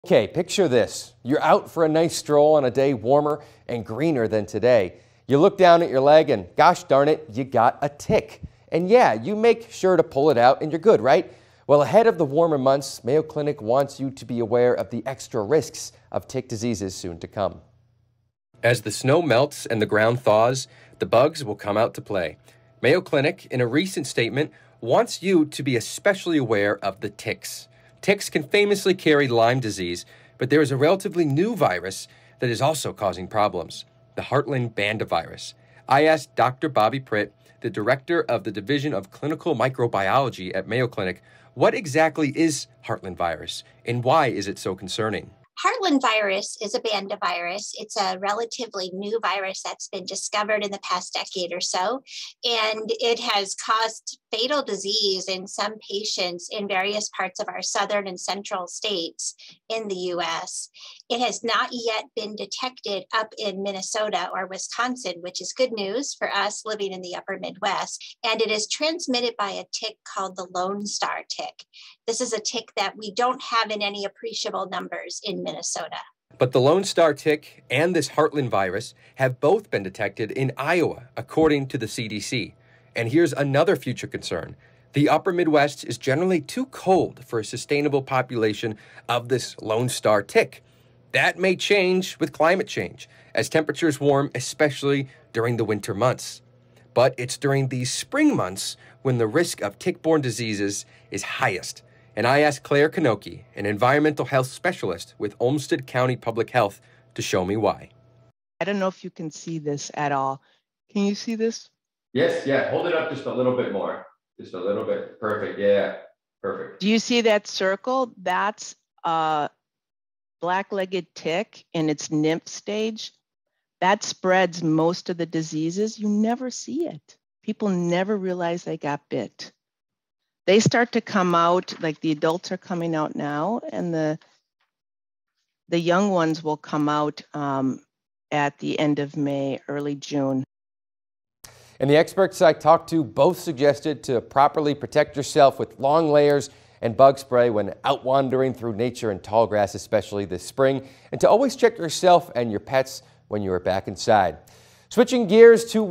Tick Concerns This Spring
Mayo clinic is reminding folks to be aware of ticks and tick-borne diseases this Spring season